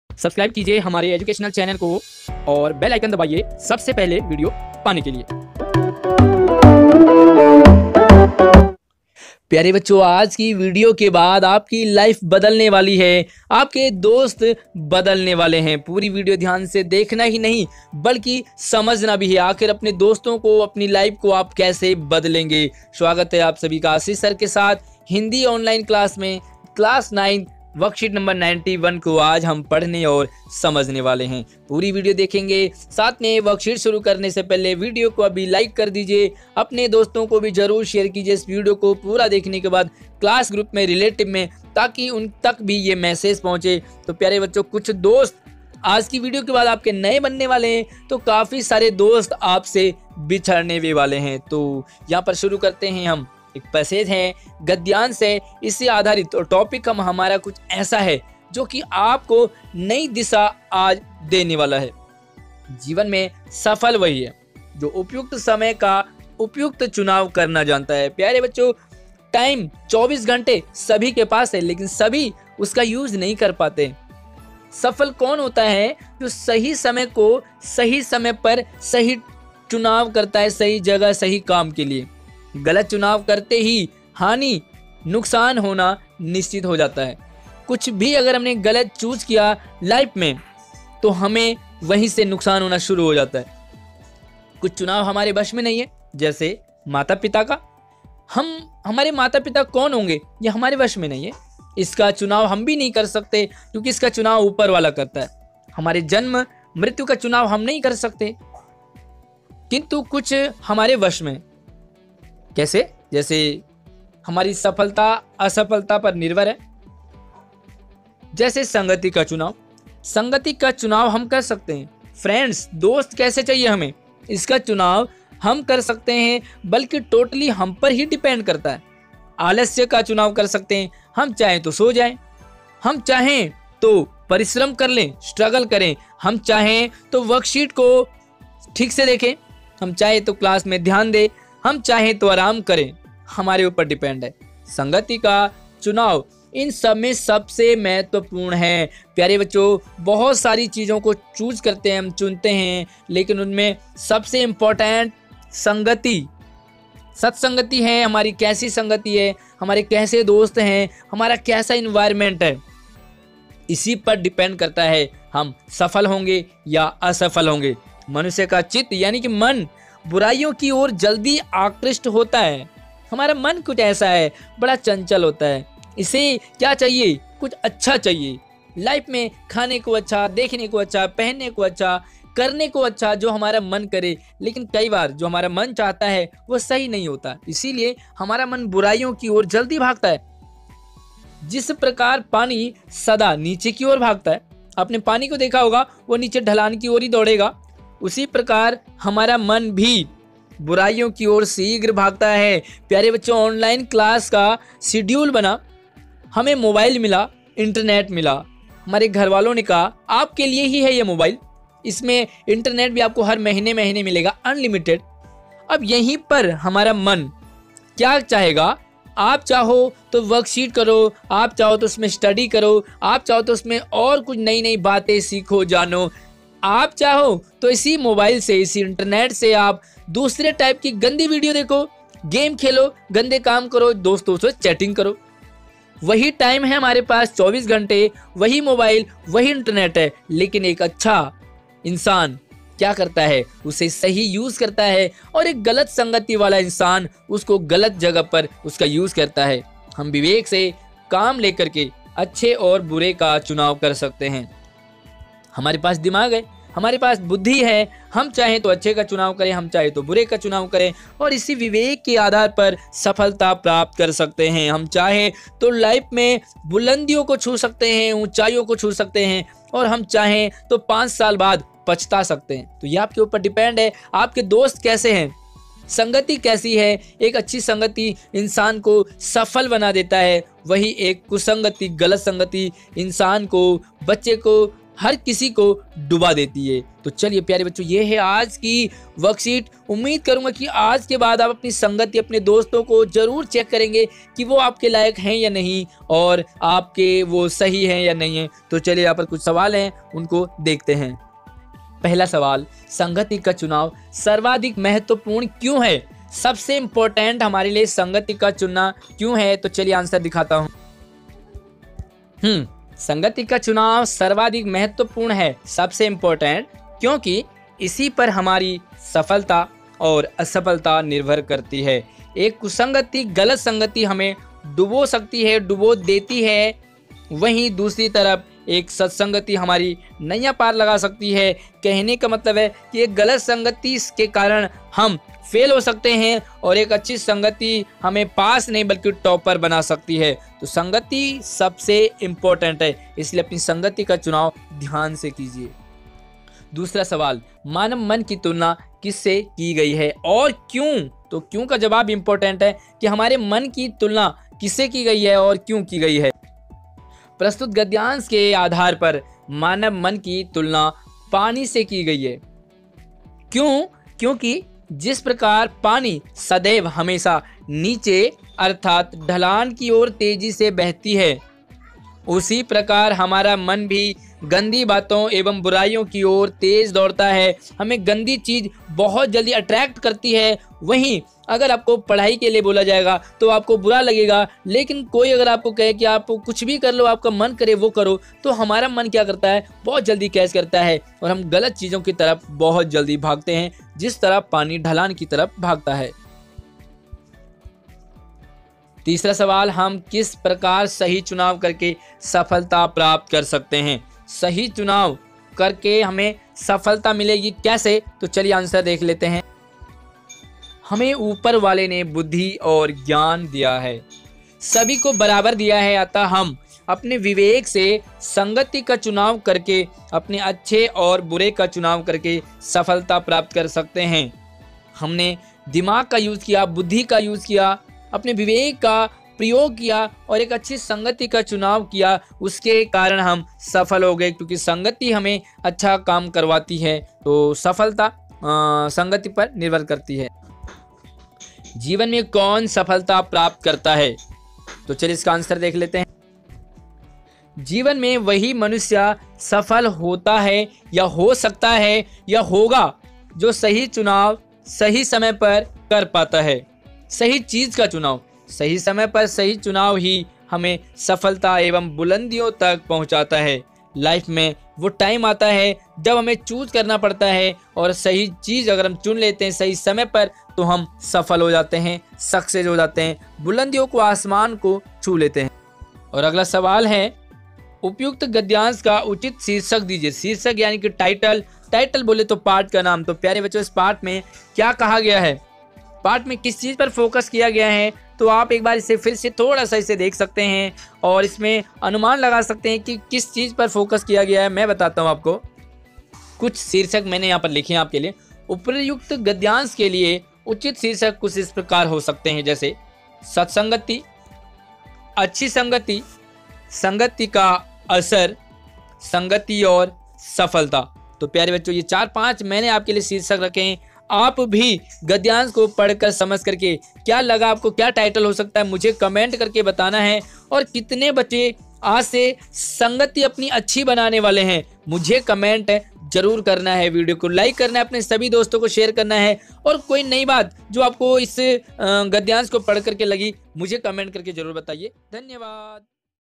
Hindi_worksheet_91 class 9.अपठित गद्यांश संगति का प्रभाव unread passage based question 26 फरवरी
9th क्लास की सभी पुरानी वर्क शीट के लिए यहां लिंक पर क्लिक करें -👇👇 worksheets समझने के लिए इस लिंक पर क्लिक करें 👇👇

प्यारे बच्चों & टीचर्स इस वीडियो में आपके लिए हिंदी कार्यपत्रक 91 है, तो आप इस वीडियो को पूरा ध्यान से देखे और समझे। धन्यवाद।